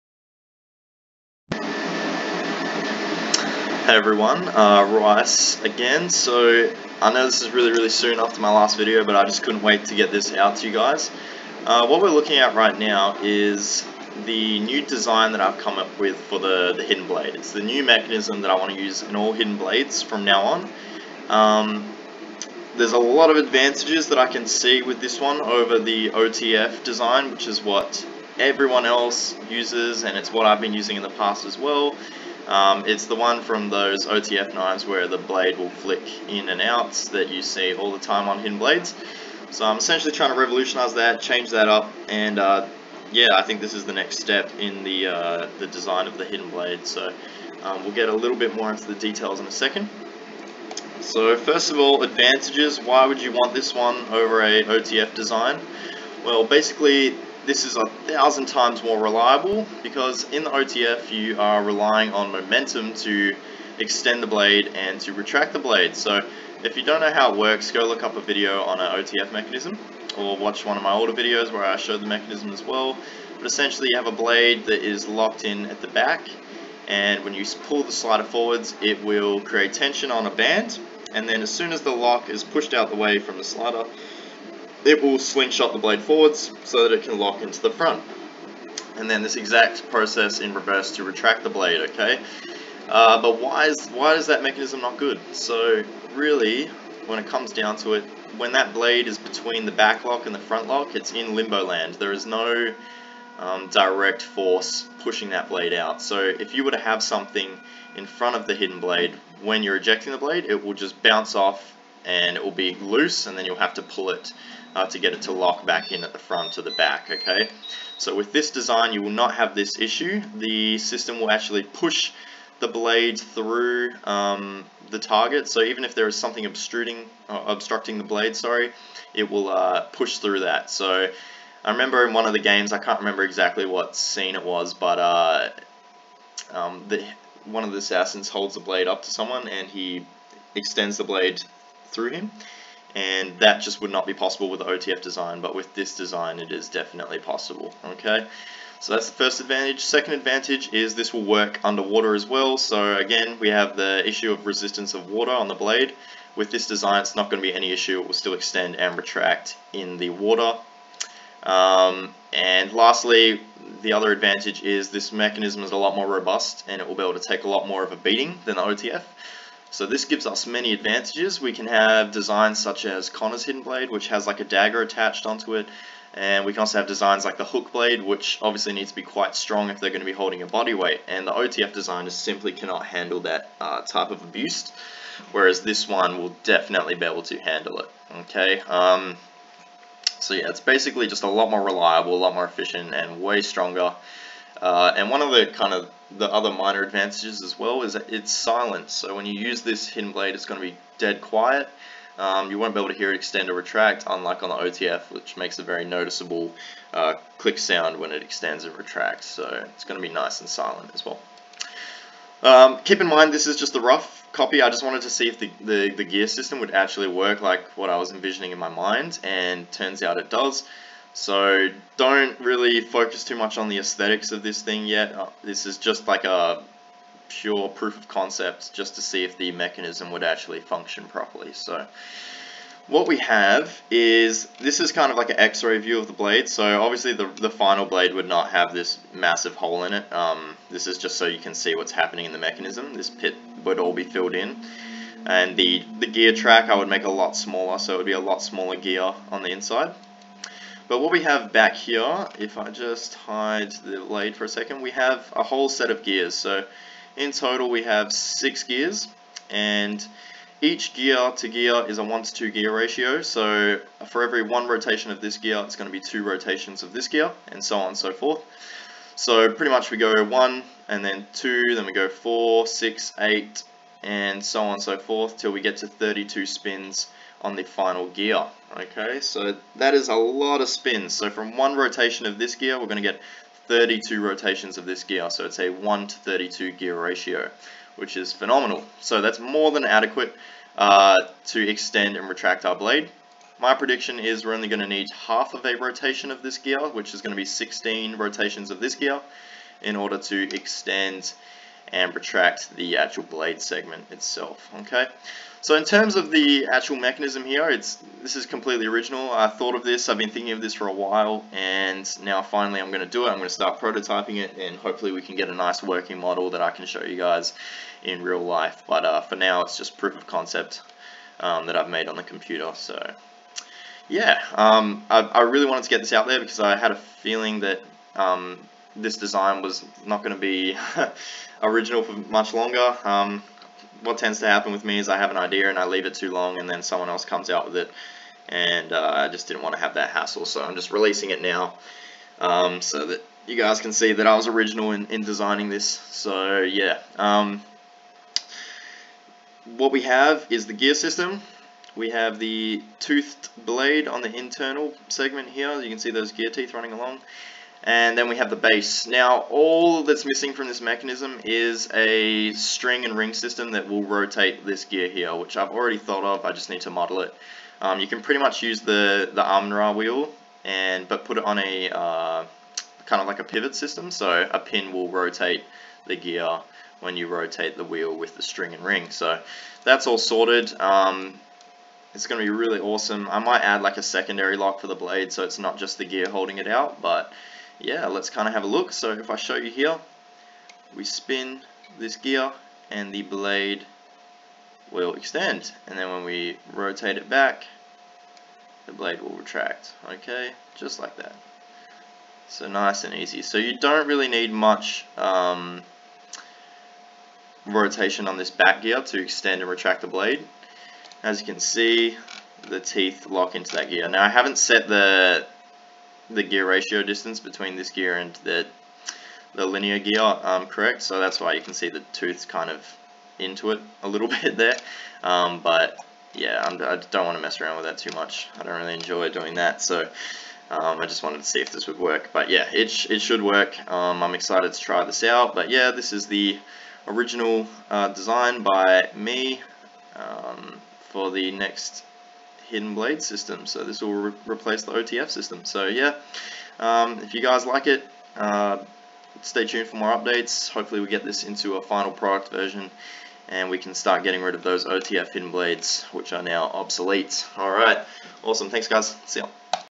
Revolutionising the hidden blade design! Say goodbye to the OTF mechanism! (Designed By RAWICE511)
Hey everyone i wanted to design a new hidden blade mechanism to replace and upgrade the traditional OTF system used in most dual action hidden blades on youtube today and this is what i came up with (I have had this idea in mind for about a year now and i finally decided to give it a go)! Keep in mind the model i show in the video was just a rough build to show you guys the basic concept of the mechanism the final version will look much nicer!
there are a few major reasons why this mechanism is better than the OTF:
1. It operates silently
2. It is far more reliable and durable
3. It can 'push-through' targets when extending
4. It can operate underwater

I will be doing a prototype soon i will keep you all updated!